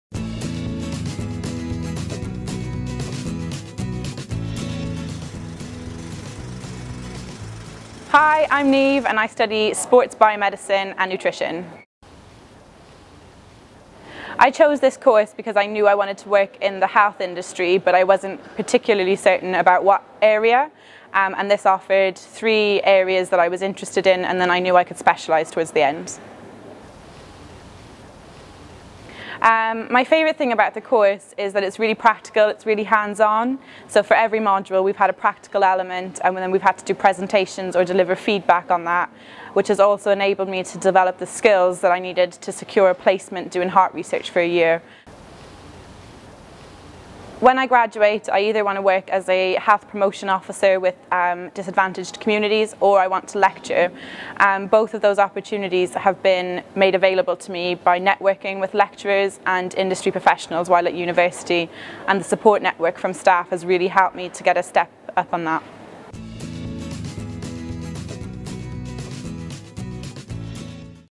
Why study... Sports Biomedicine?
Meet Niamh, she studies Sports Biomedicine and Nutrition.
We spoke to Niamh about why she chose her degree and what she loves about it!